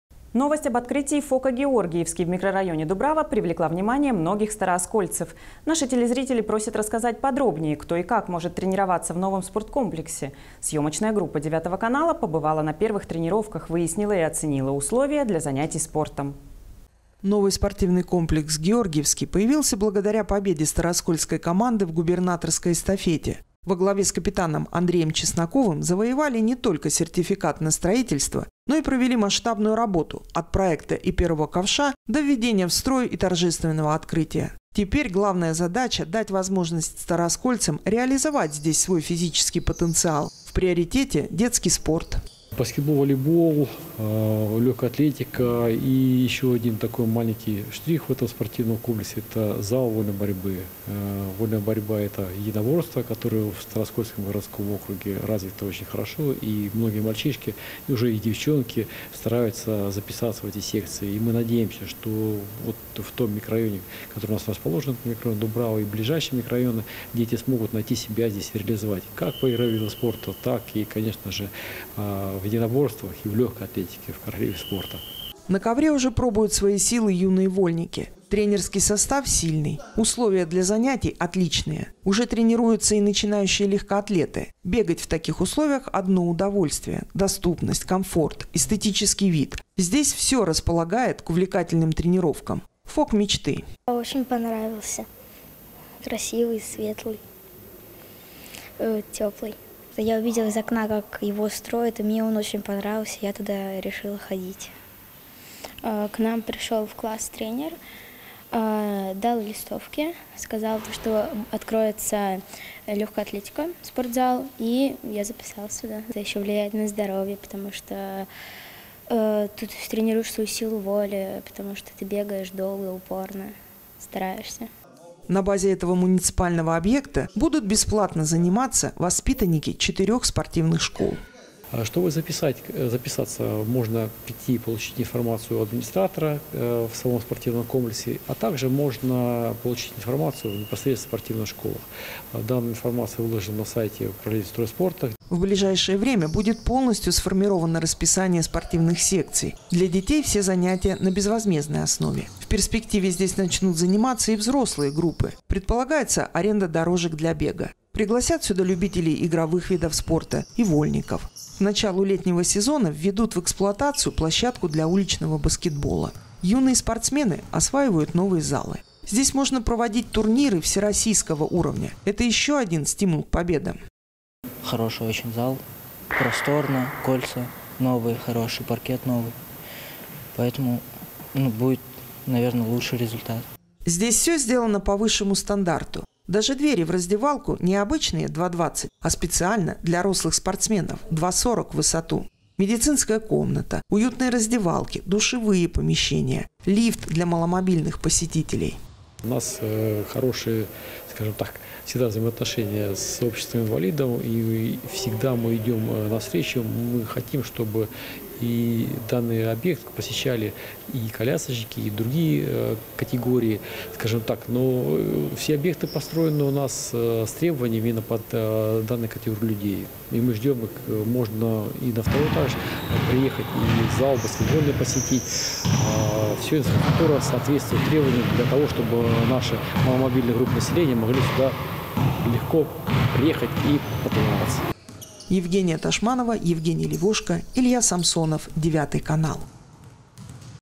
Место силы: в мкр. Дубрава открылся новый ФОК «Георгиевский»
Новый спортивный комплекс «Георгиевский» появился благодаря победе старооскольской команды в губернаторской эстафете. Во главе с капитаном Андреем Чесноковым завоевали не только сертификат на строительство, но и провели масштабную работу - от проекта и первого ковша до введения в строй и торжественного открытия. Теперь главная задача - дать возможность старооскольцам реализовать здесь свой физический потенциал. В приоритете - детский спорт.